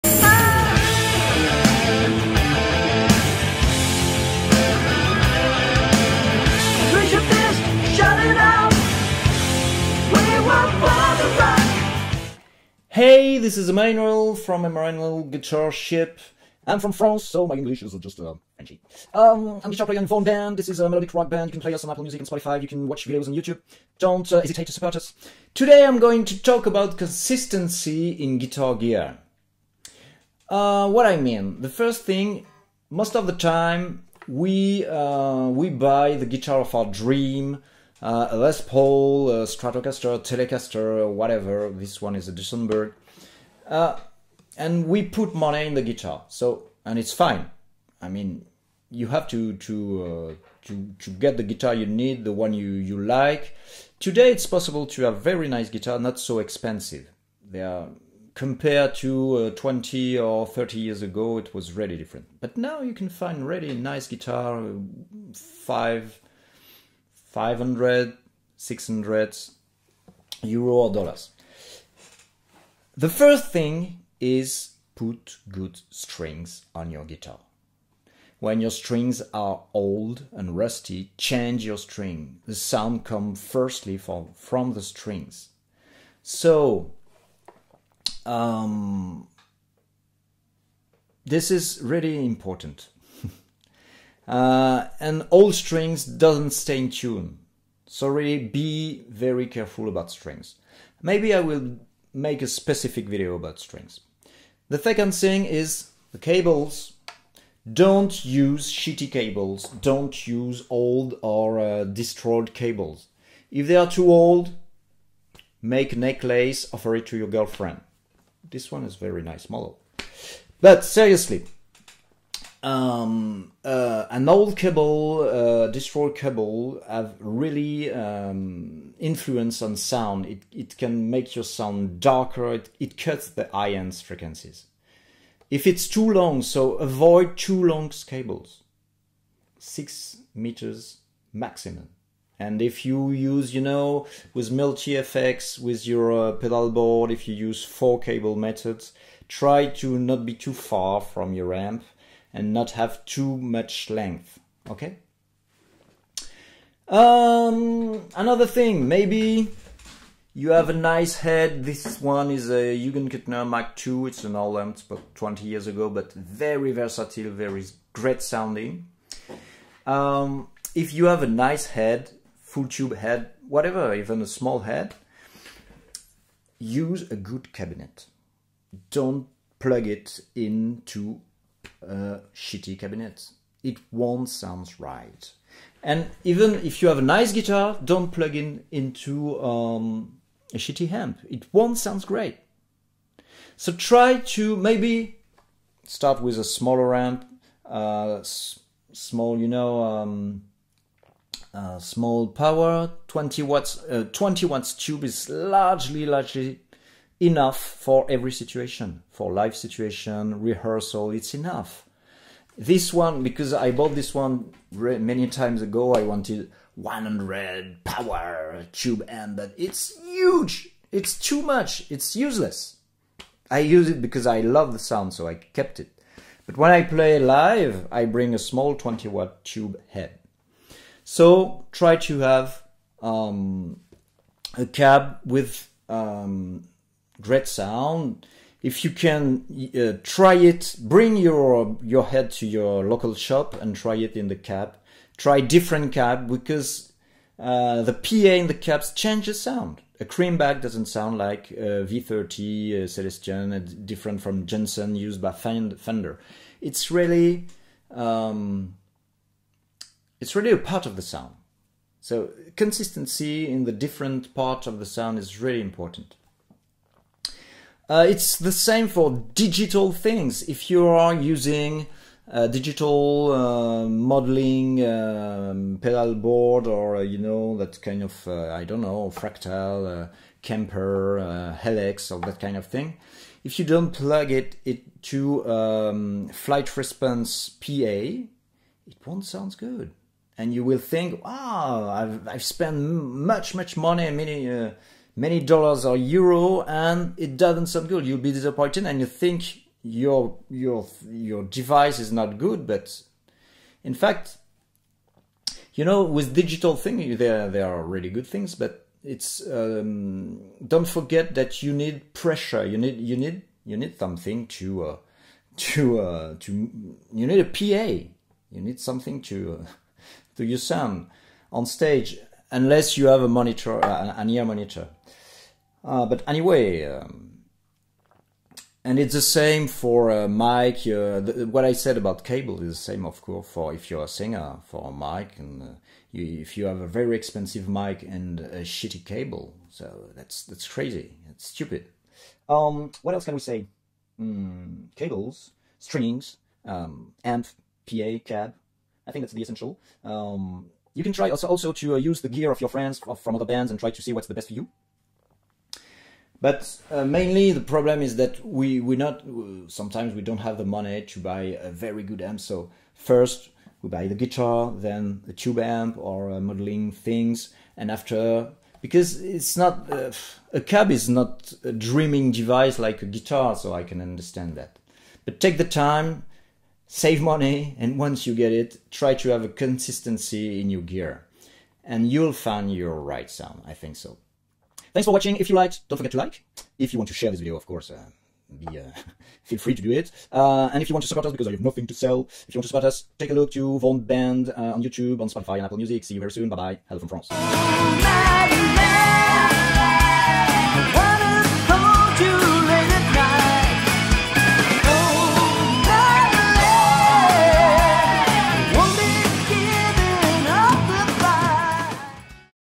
CONSISTENCY IN GUITAR GEAR
I talk about the importance of consistency in your guitar gear : strings, cables, amps, cabs, PA...